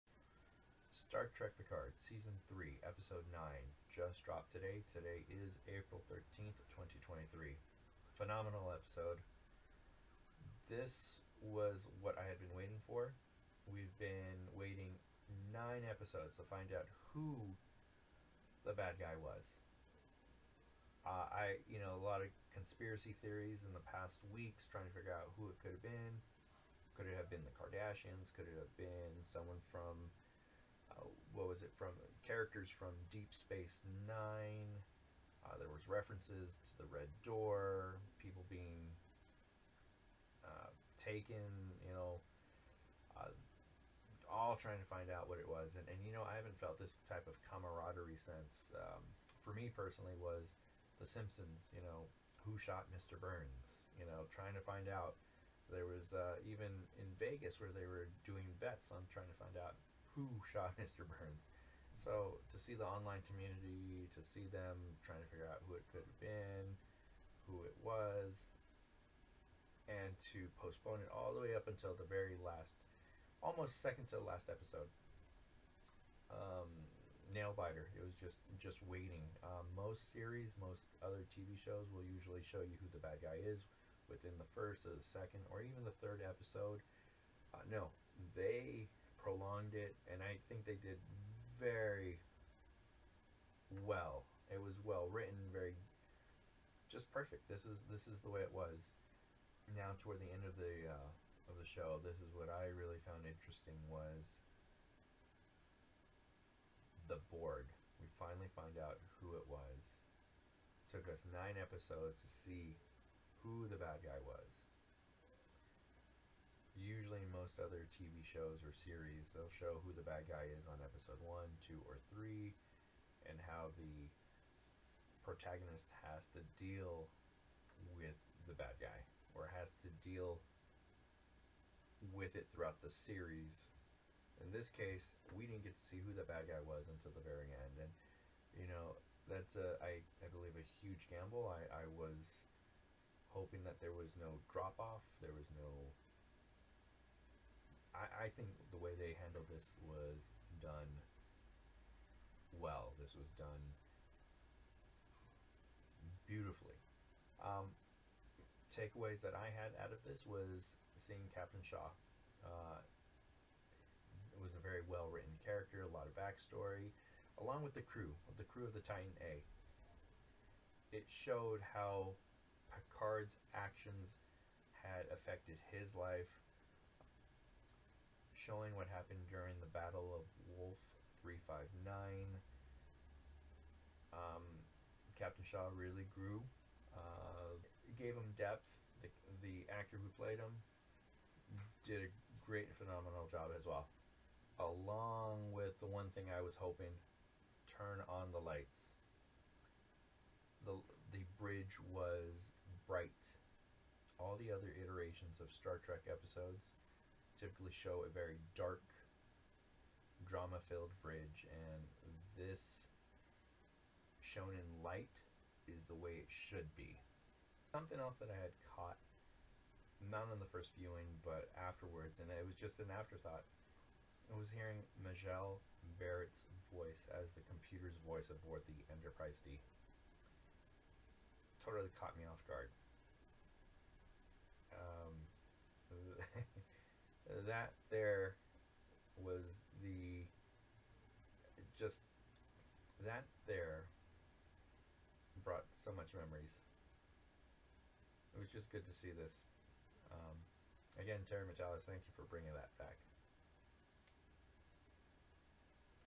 Spoilers Star Trek Picard S3E9 Thoughts!
Very quick thoughts about todays episode of Picard. I don't usually post videos about myself, but I wanted to join in with anybody who has fondness for Star Trek!